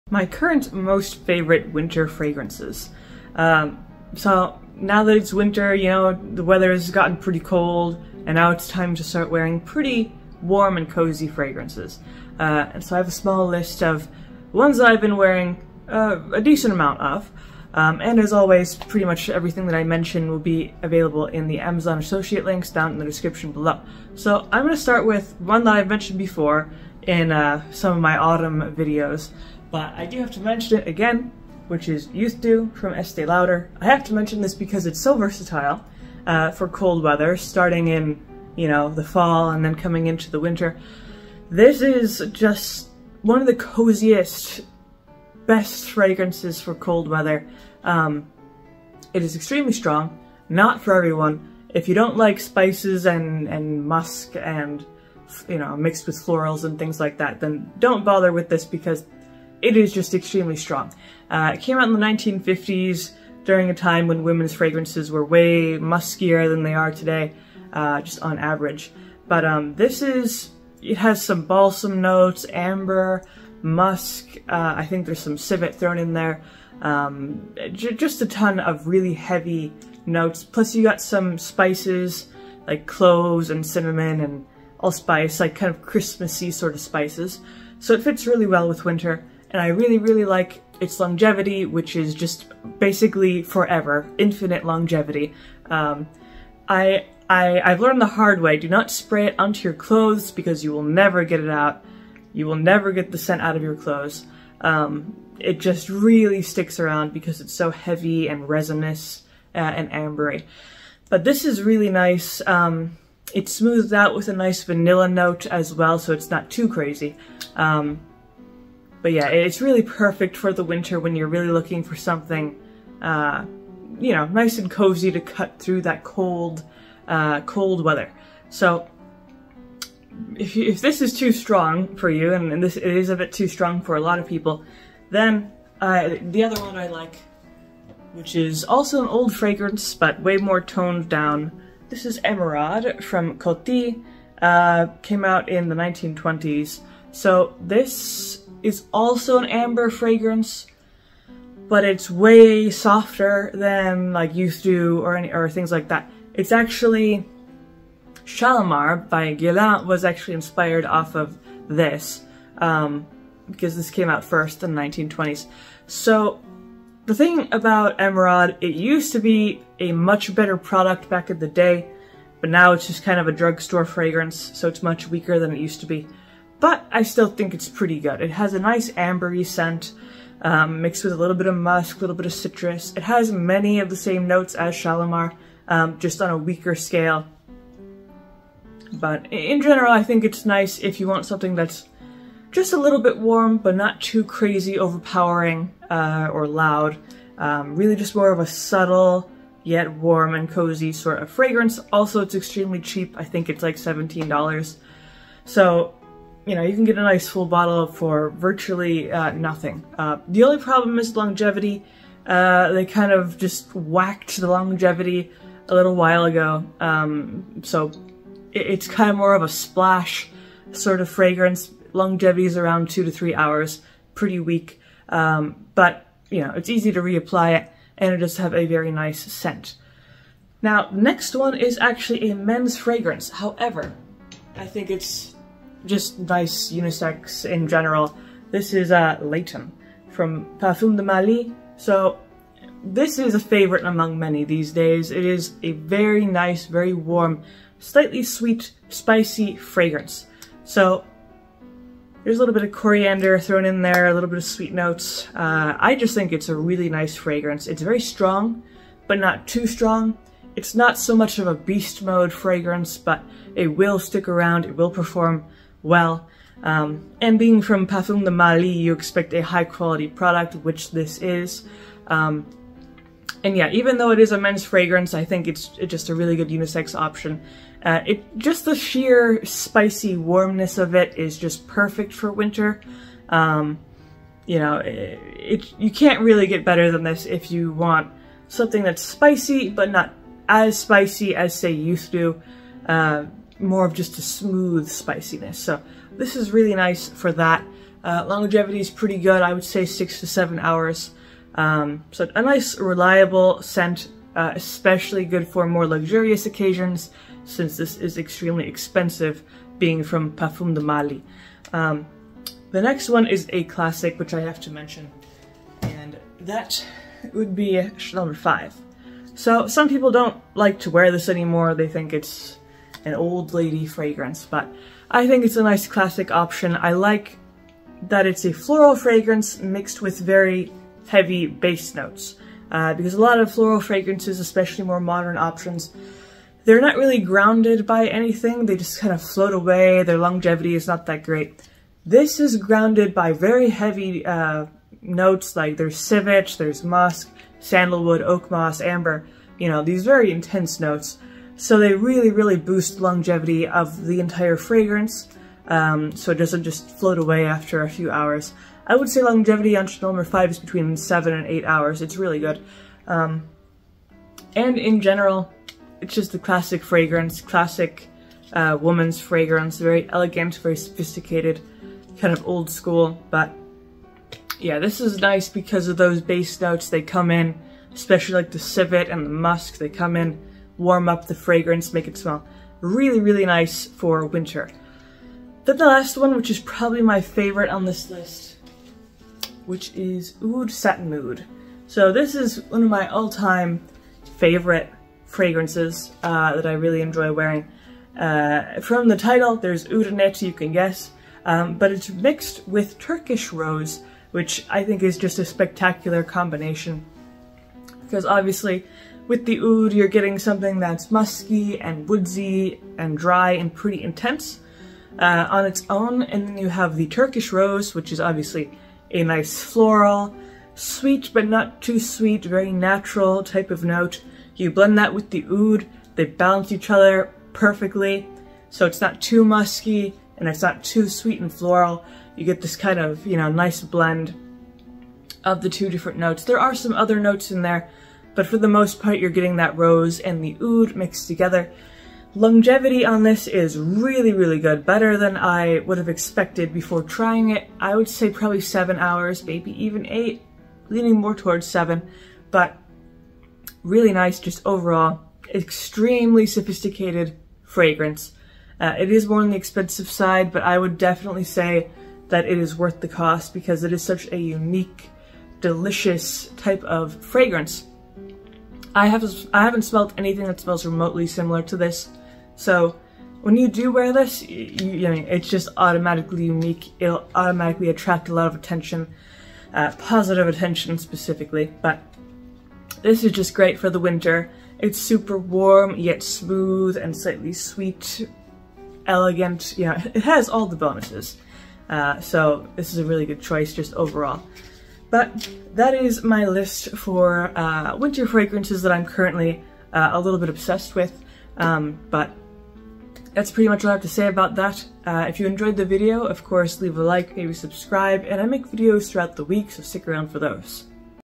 MY CURRENT WINTER FRAGRANCE OBESSIONS (CAN'T LIVE WITHOUT THESE)
These are my current winter fragrance obsessions:
Youth Dew Estee Lauder
Emeraude by Coty
Layton Parfum de Marly
Chanel No 5
Oud Satin Mood

🎨Etsy shop